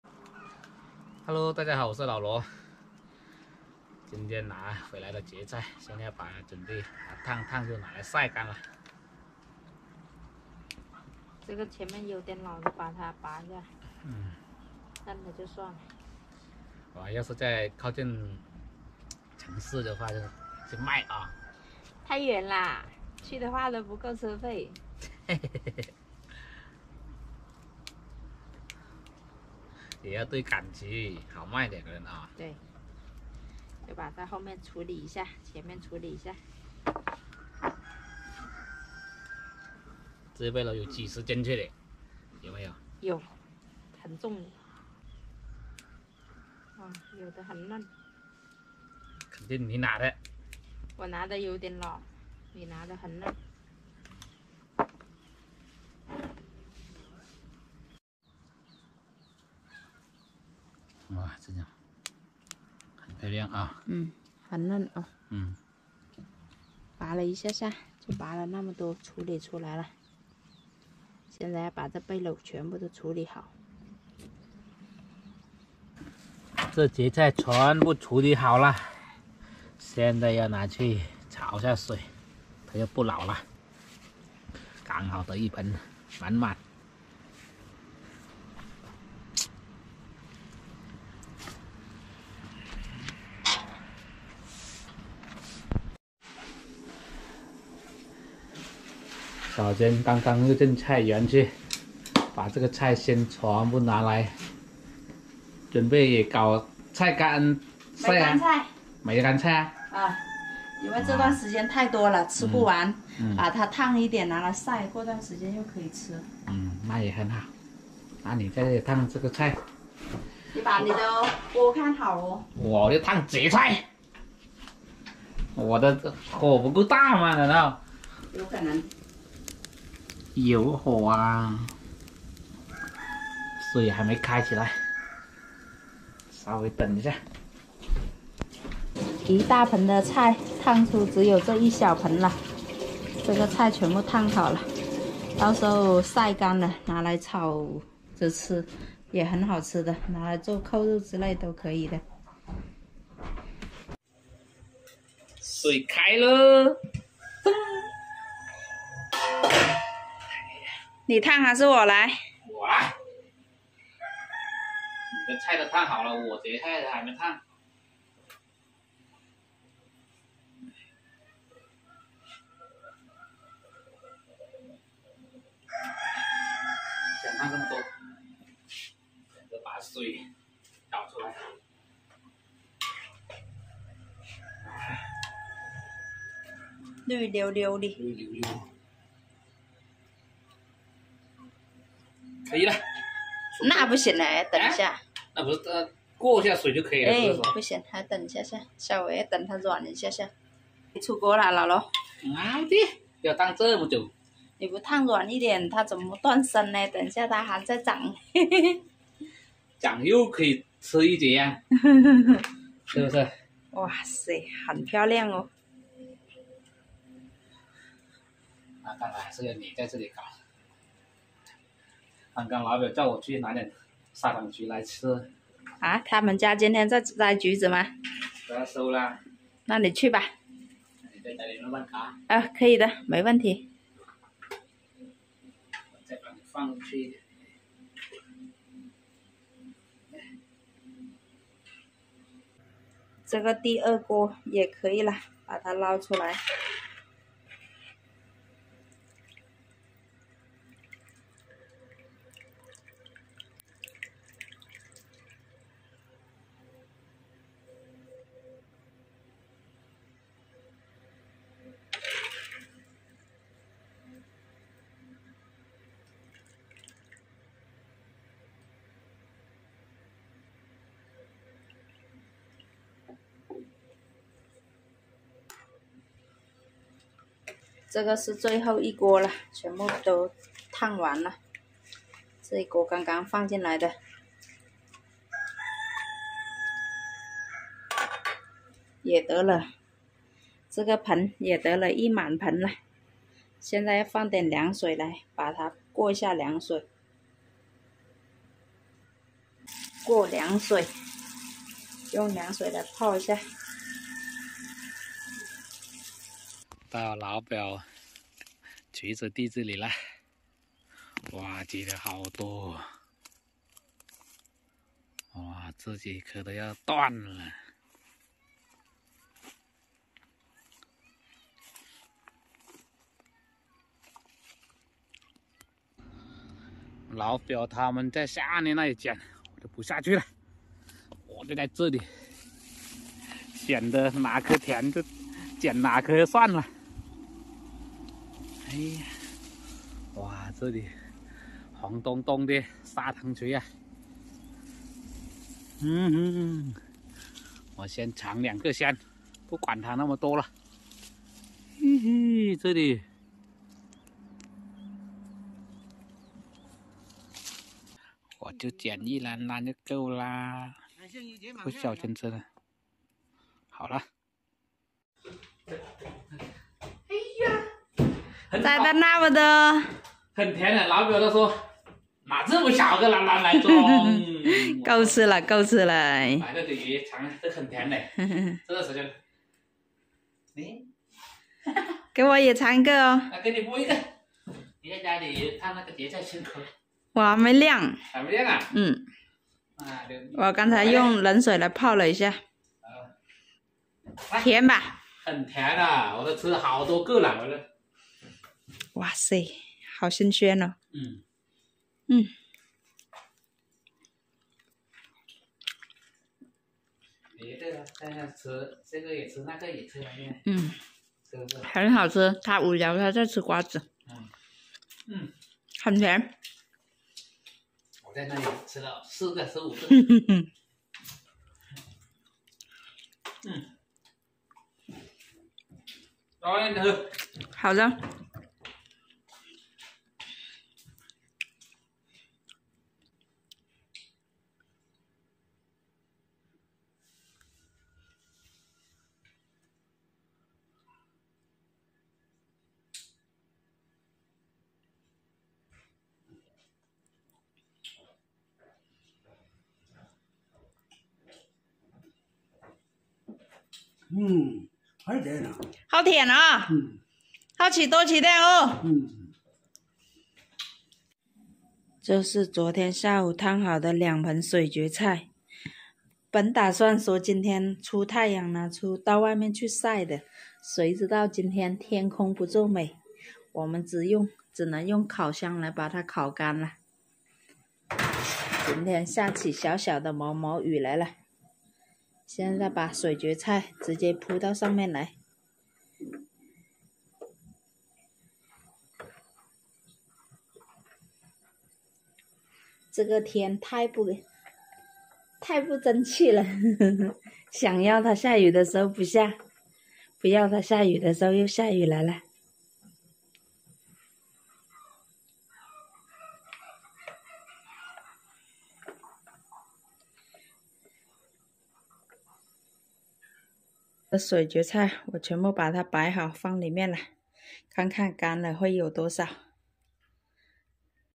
摘回來的水蕨菜用來焯水曬乾，製作野干菜，這樣的做法你們有吃過嗎？
請注意，本影片內容完全符合法律規定，不會涉及對動物的不當對待！訂閱老羅~
大家好，我是老羅
分享戶外生活，體驗搞野樂趣
更多有趣日常等你來看~